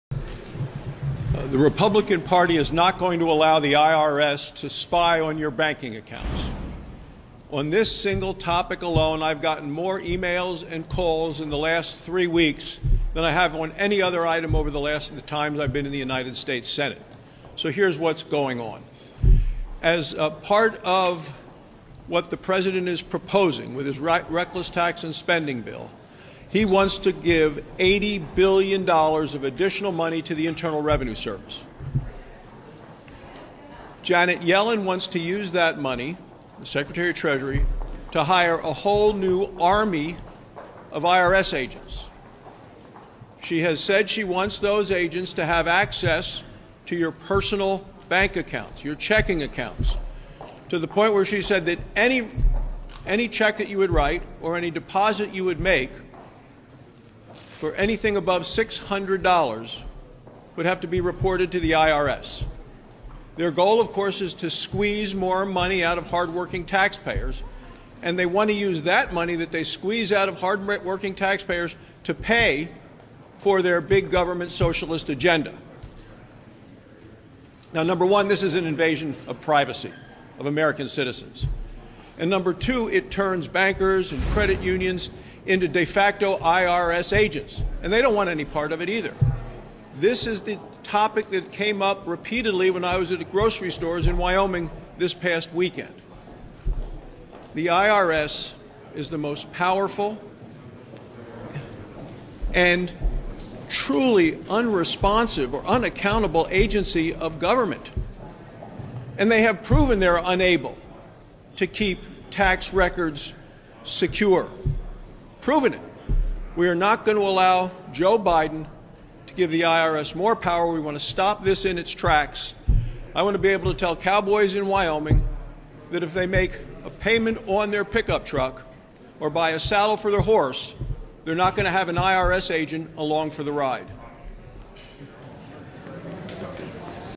IRS Will Be Able To Spy On Your Bank Accounts
Sen. John Barrasso (R-WY) warned against Biden’s latest proposal that would include requiring banks to turn over to the Internal Revenue Service bank account information for all accounts holding more than $600.

Barrasso says, "The IRS is the most powerful and truly unresponsive or unaccountable agency of government, and they have proven they are unable to keep tax records secure. We are not going to allow Joe Biden to give the IRS more power."